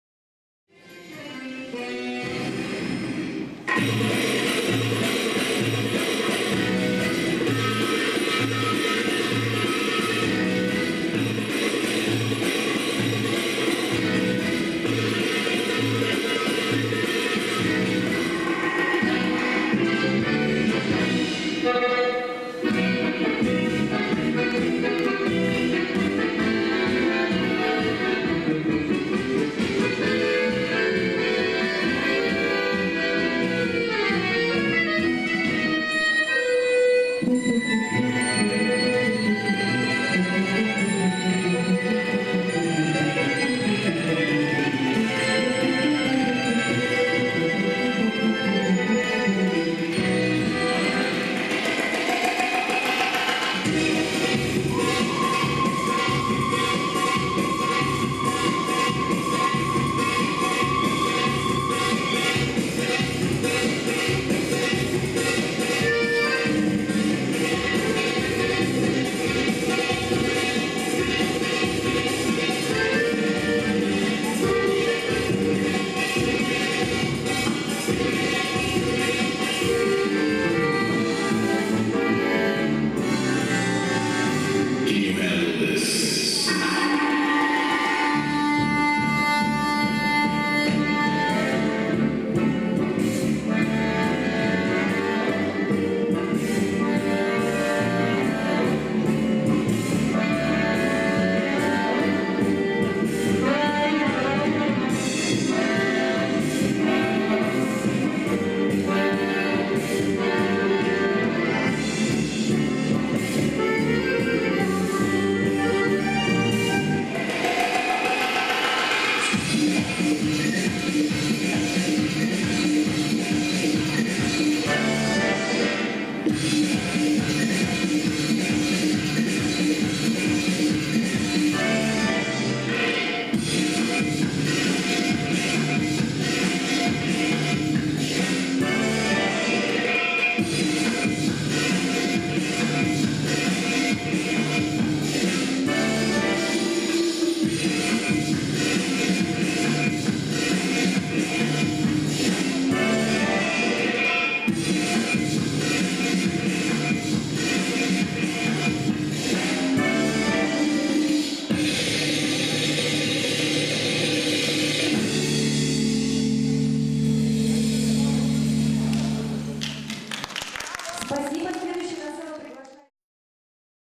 «Jamilina & Jaipur» ⊰⊱ Ukrainian Bellydance Championship '14.
«Джамилина+Джайпур» (г.Днепропетровск) на Втором Украинском Чемпионате по восточным танцам  г. Днепропетровск, КДЦ "Днепро Пресс" 19.04.14.
(Формейшн шоу) 6,54 .